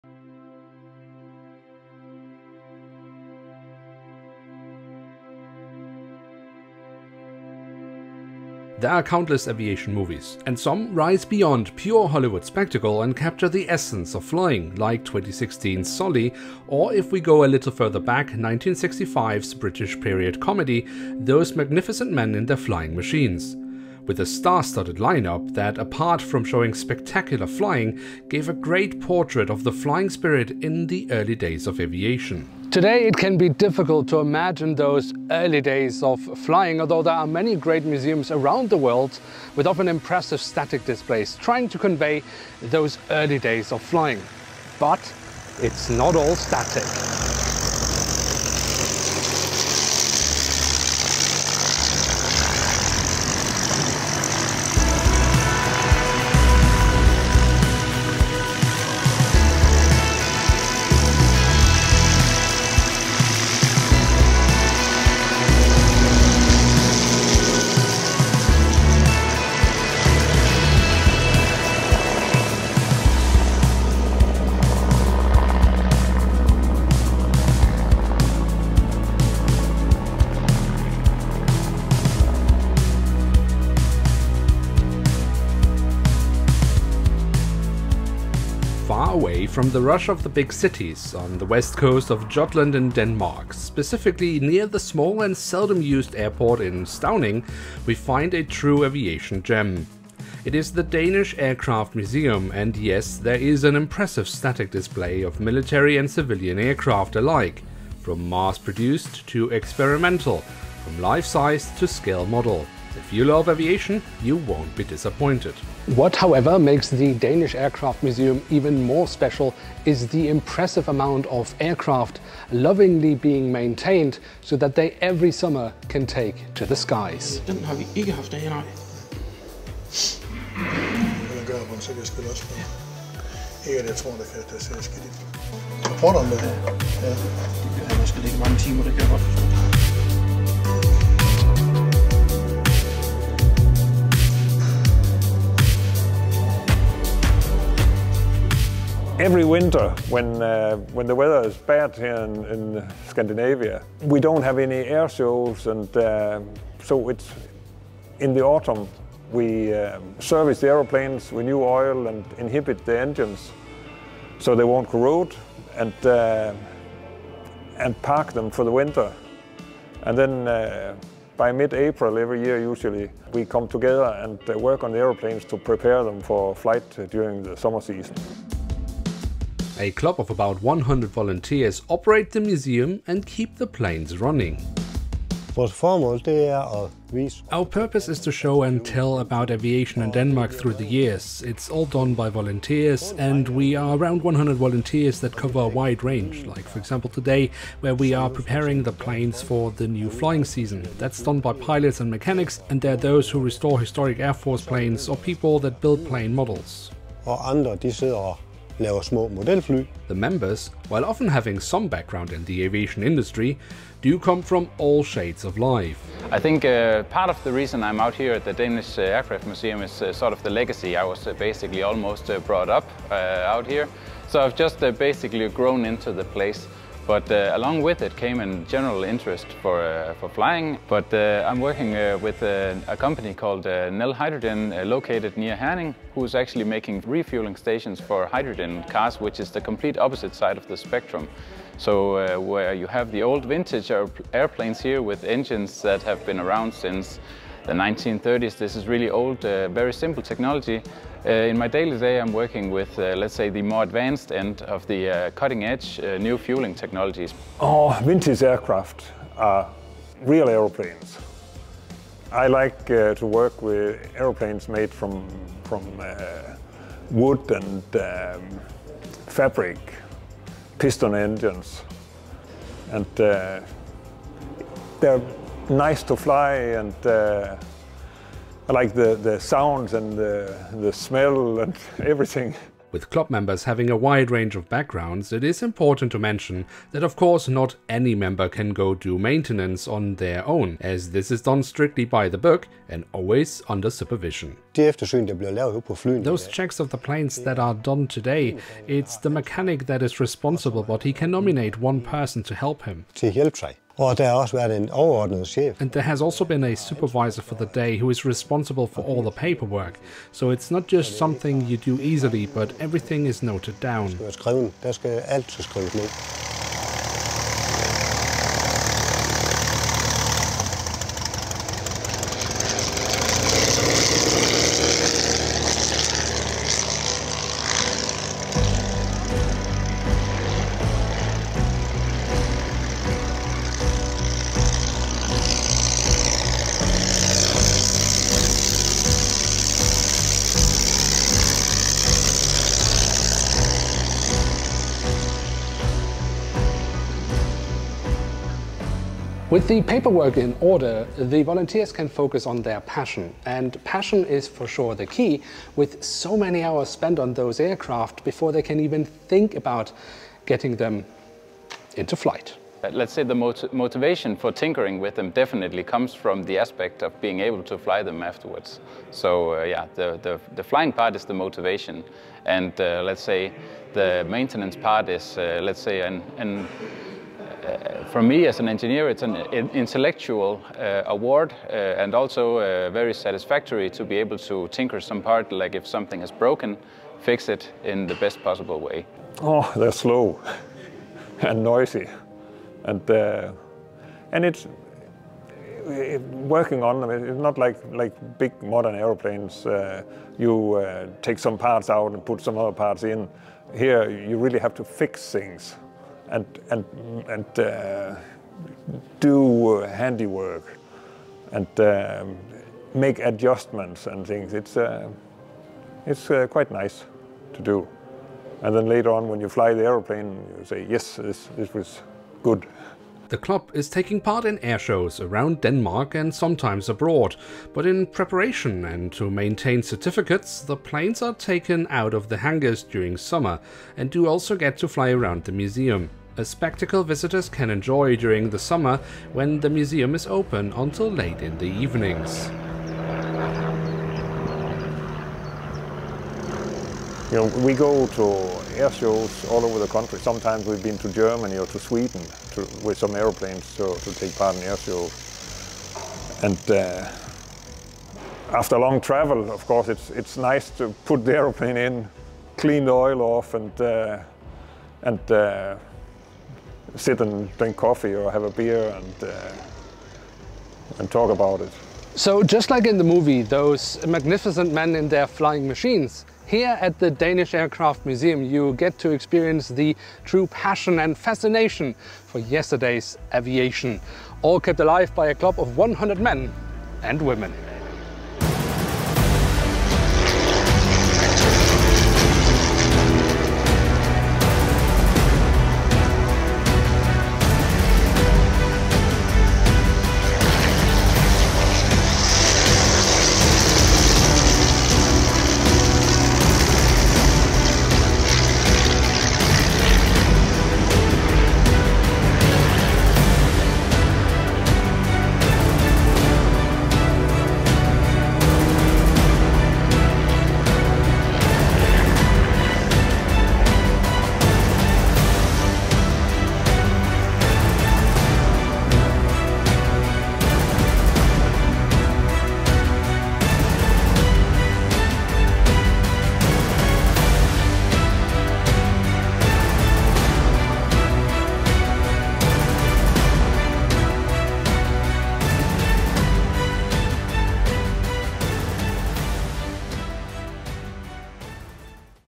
Those Magnificent Flying Machines | BongoPlanes Does Aviation Documentary
Those Magnificent Flying Machines is a new aviation documentary from BongoPlanes Does Aviation where we take a look at a true aviation gem. The Danish Aircraft Museum situated near the small danish airport in Stauning on the west coast of Jutland. A museum that with an impressive static display shows aviation in Denmark trough time but what makes the museum special is the amount of aircraft still year after year take to the skies.

My diecast model aviation channel: @bongoplanes